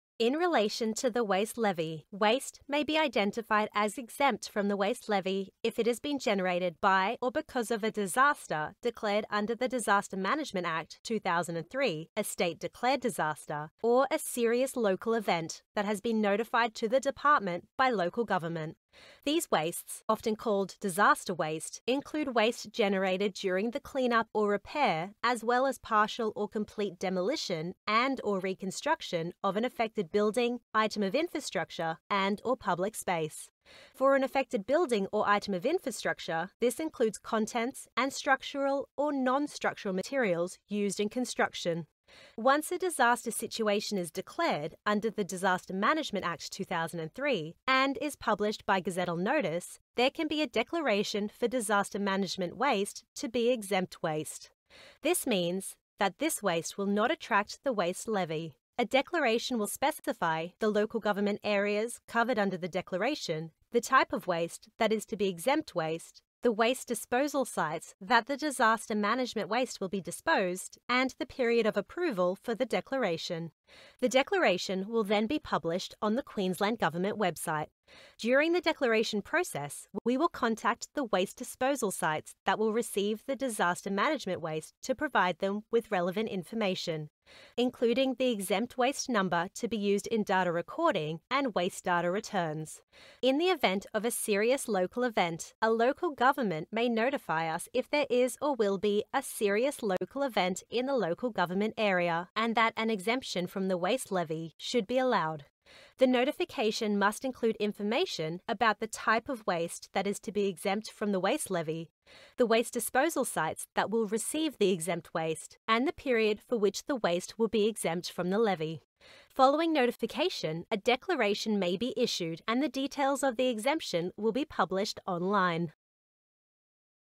Waste industry – Waste levy
This webinar is for landfill operators and explains the waste levy obligations for disaster waste.